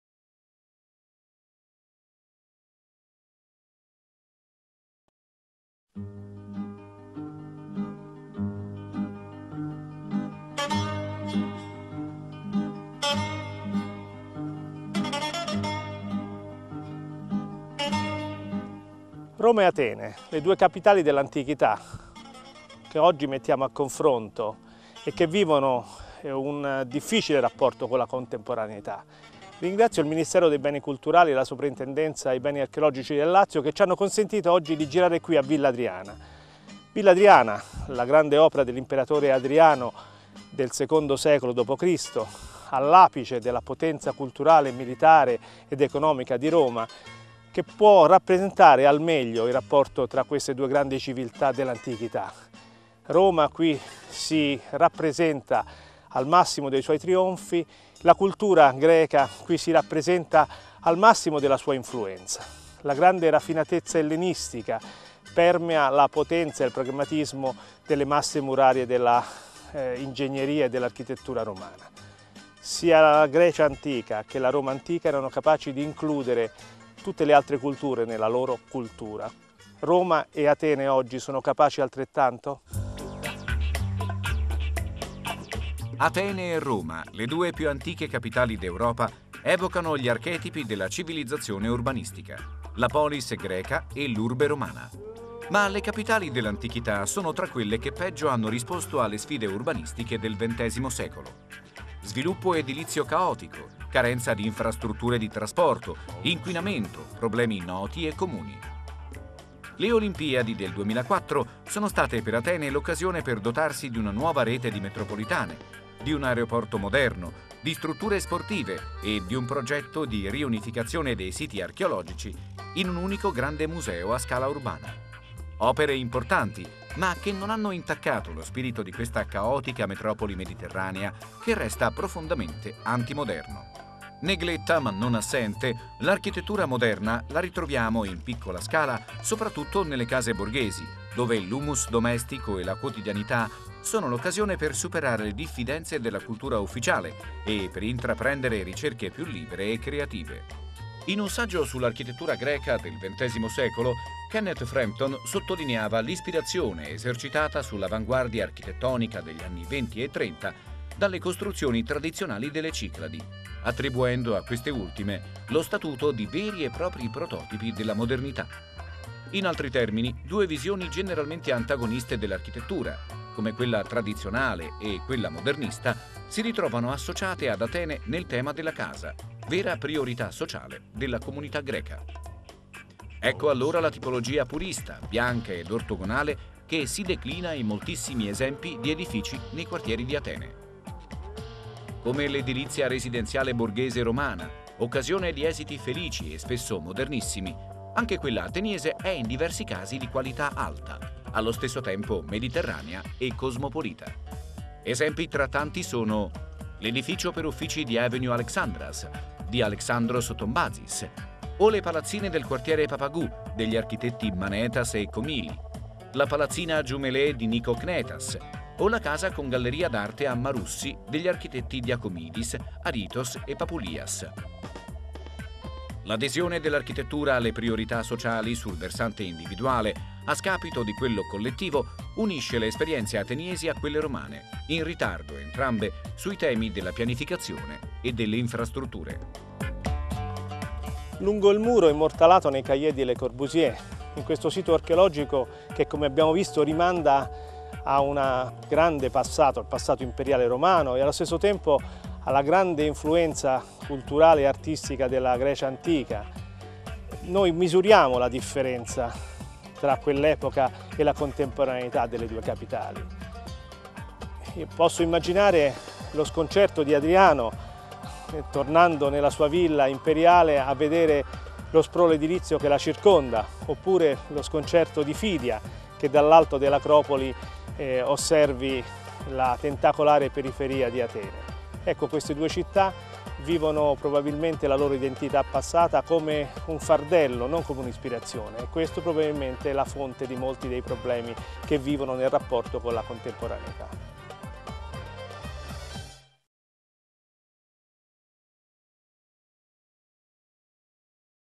Roma | Atene. Trasformazioni delle città storiche a confronto - Vivere l'architettura #6.12
La dodicesima puntata è intitolata: 'Roma | Atene. Trasformazioni delle città storiche a confronto '.

Nella clip l'arch. Andrea Giunti nella cornice di Villa Adriana a Tivoli.